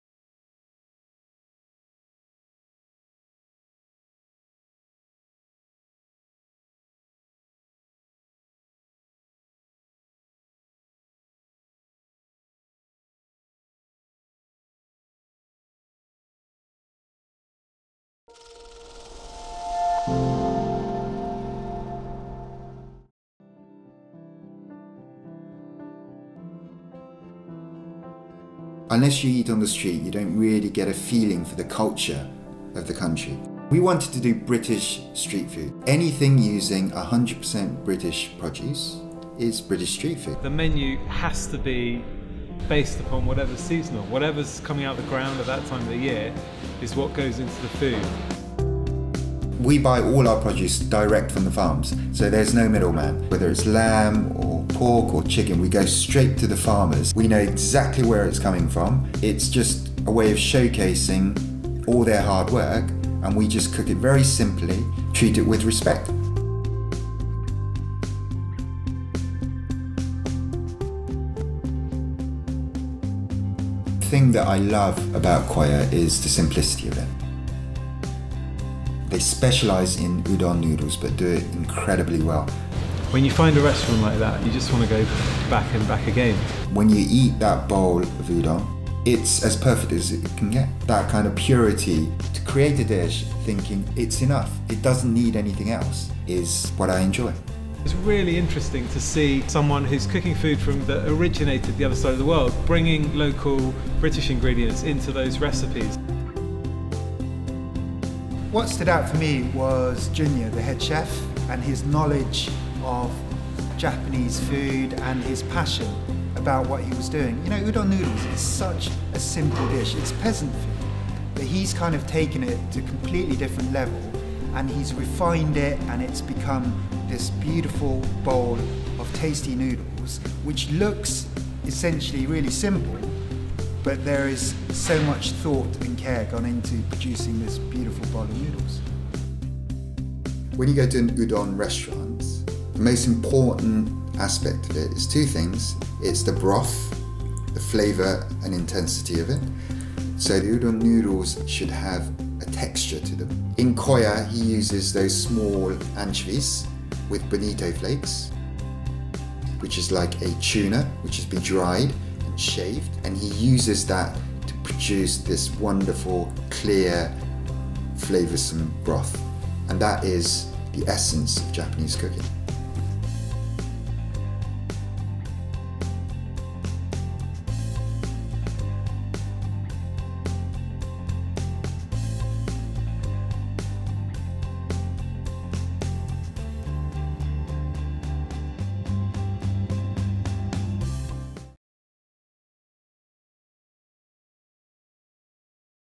Street Kitchen chefs Jun Tanaka and Mark Jankel recommend Koya
Where do chefs go to eat? Jun Tanaka (former head chef of Pearl) and Mark Jankel (former executive chef of First Restaurant) are the founders of Street Kitchen. They have a series of food vans across the capital that serve up gourmet food for Londoners to eat on the go. They both recommend Koya, a Japanese Udon restaurant in central London that serves their favourite, freshly made noodles.